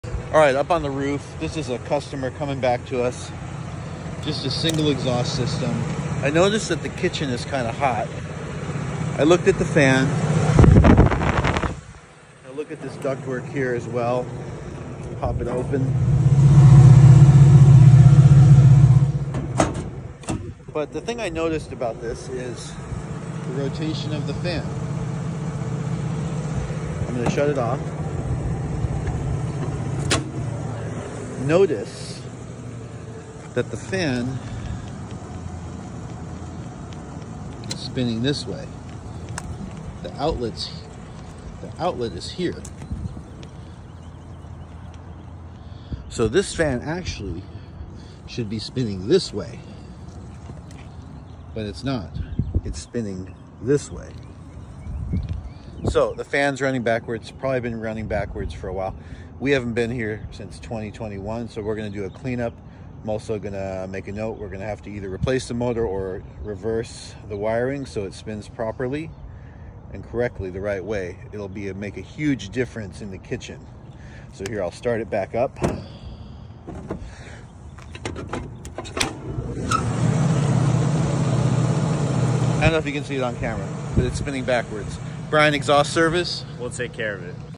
What Do You Do When A Fan is Running Backwards?
You can reverse the polarity inside the motor and it will reverse the directioon of the motor.